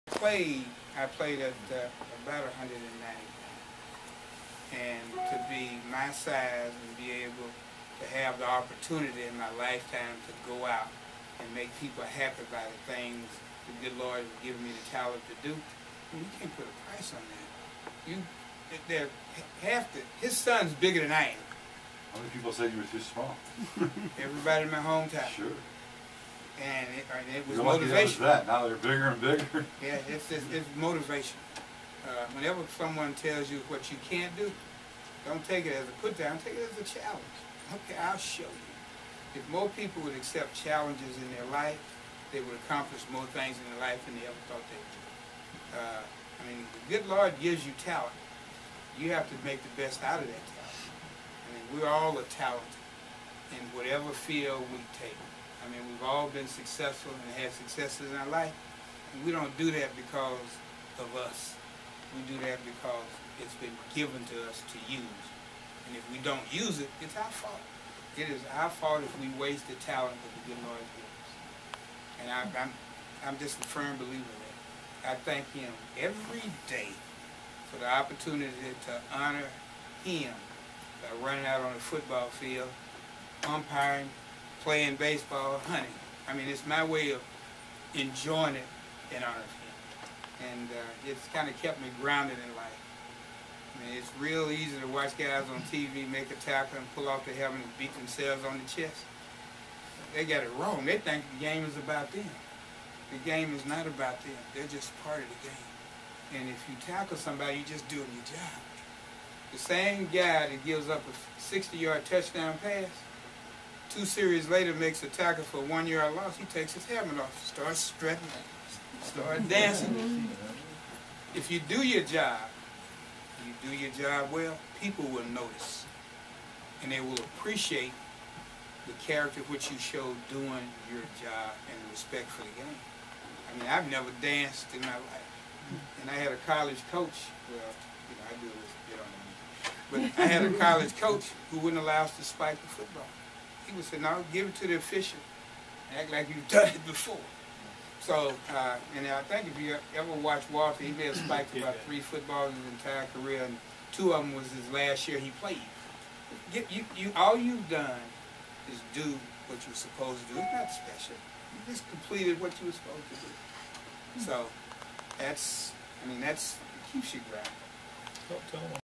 Eddie Payton book event 11.15.12, part 1
Eddie Payton, brother of Chicago Bears legend Walter Payton, talks at a book signing Nov. 15, 2012, at the Book Stall at Chestnut Court in Winnetka, Ill. Eddie is the author of the book "Walter & Me."